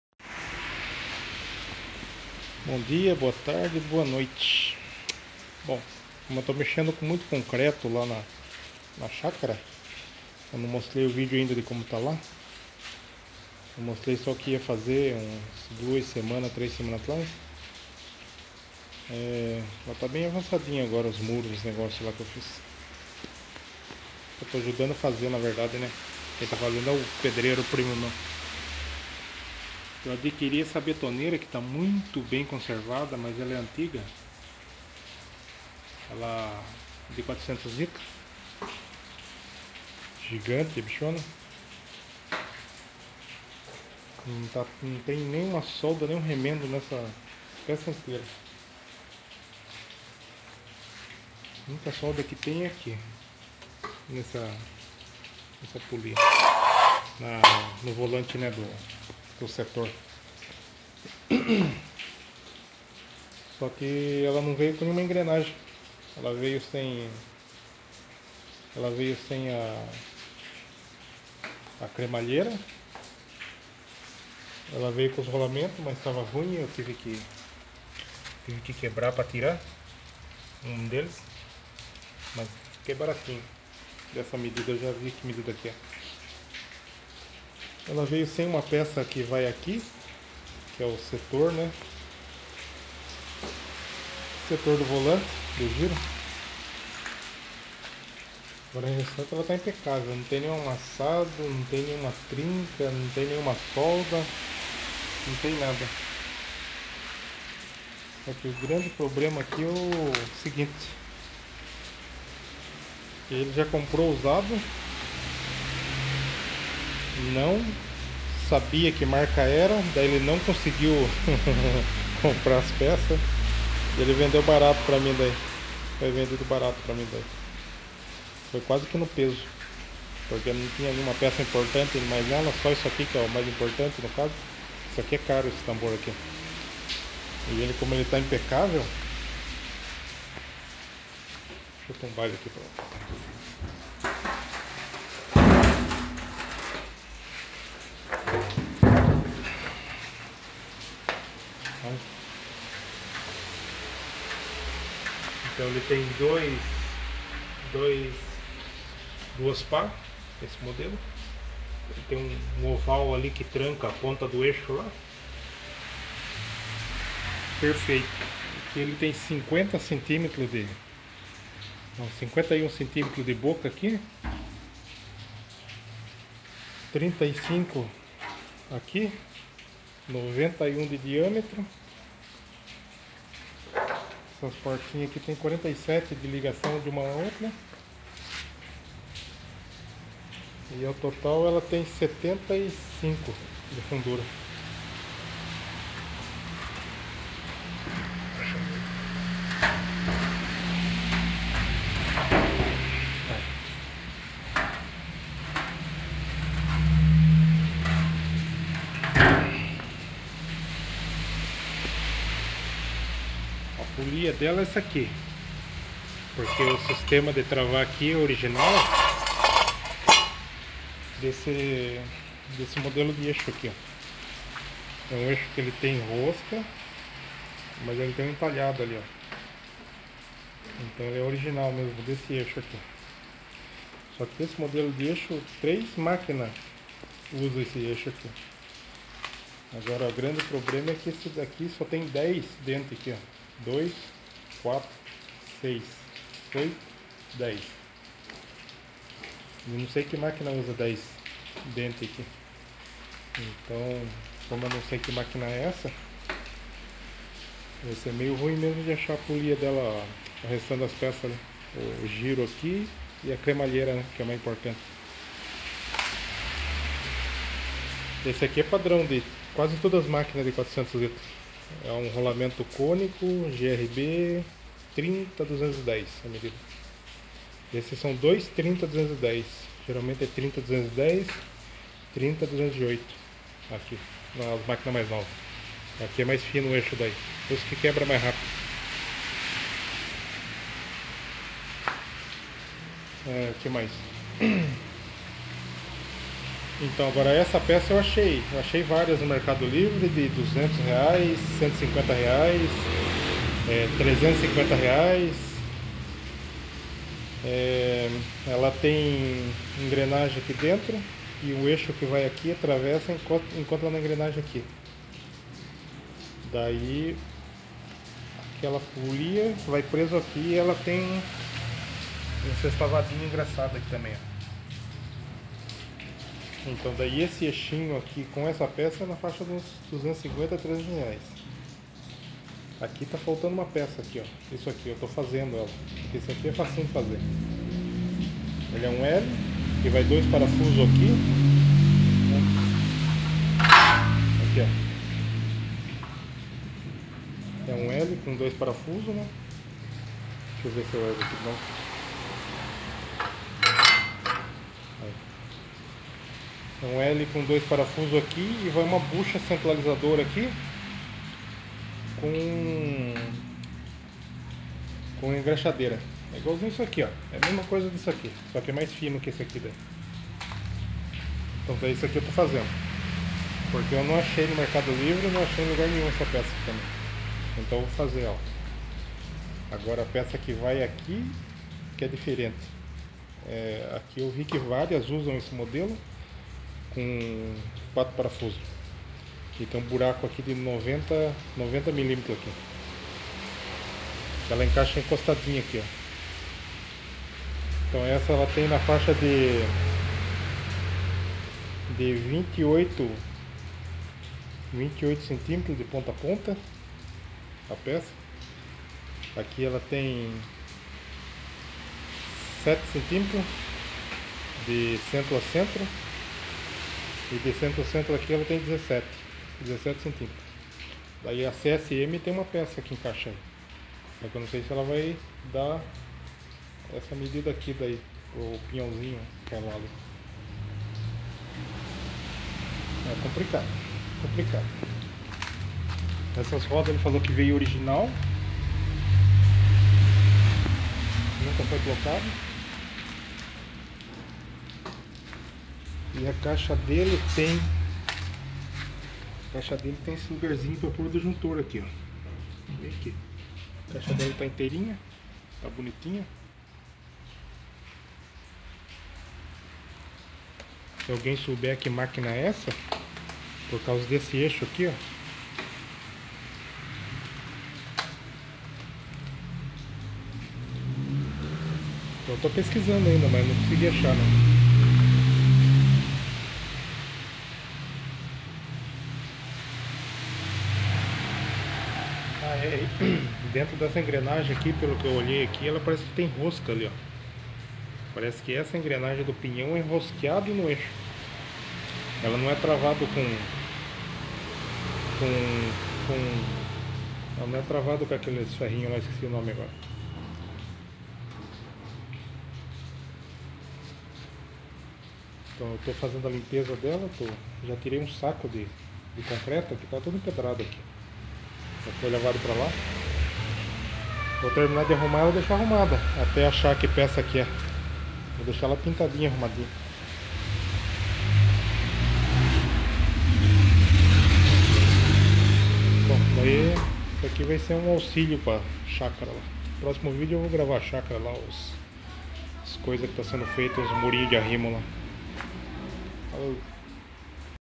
reforma da betoneira de 400 l
estou reformando  betoneira .comprei no peso basicamente
faltando engrenagem principais .mas não sei  que betoneira seria essa